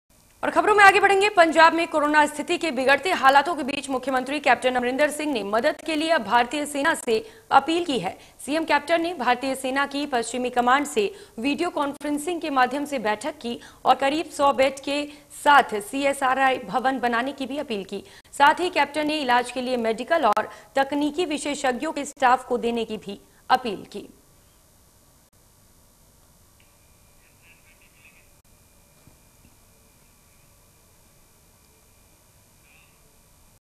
पंजाब के सीएम कैप्टन अमरिंदर सिंह ने भारतीय सेना से मांगी मदद।
पंजाब के सीएम कैप्टन अमरिंदर सिंह ने भारतीय सेना से मांगी मदद।  India News Punjab